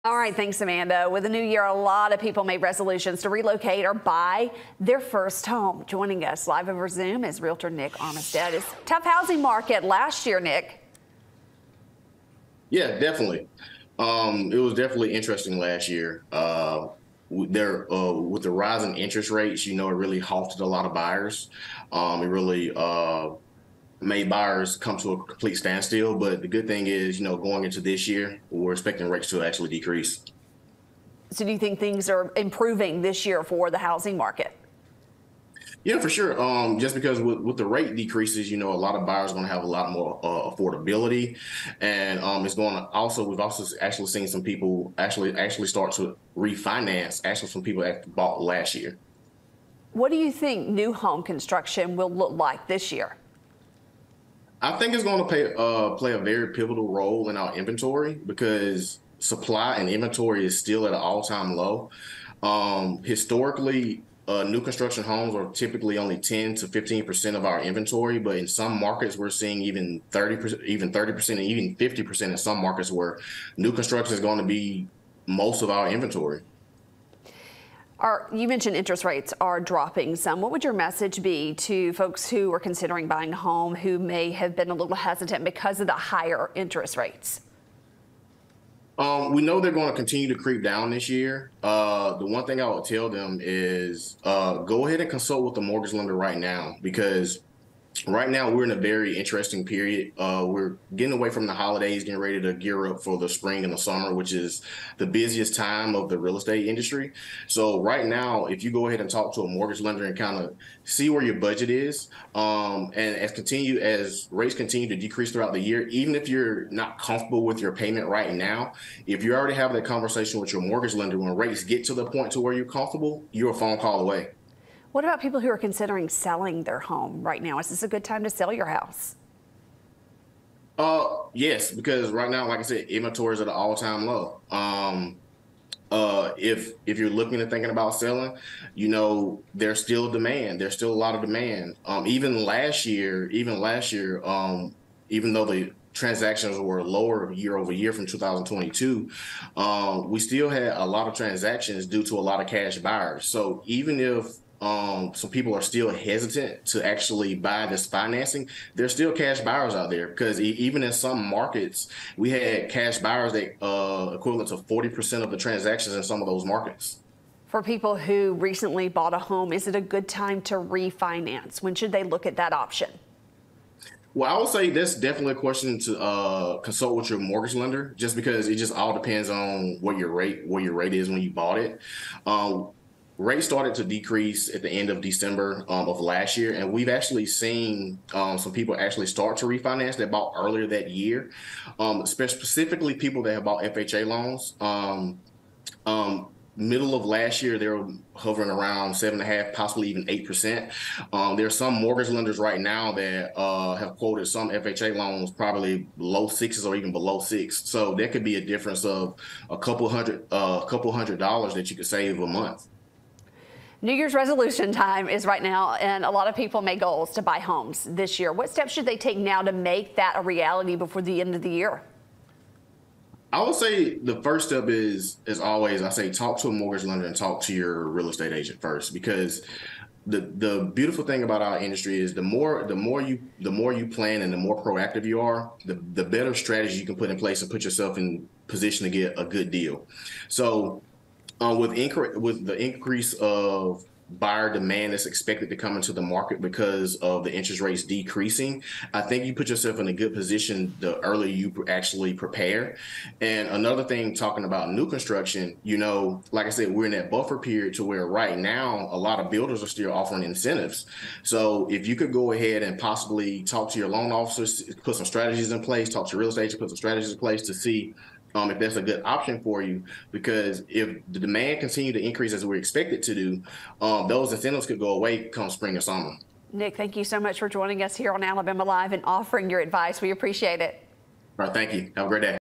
2024 housing market forecast
Realtor Nick Armistead joins us with a look at the housing marketing in the new year.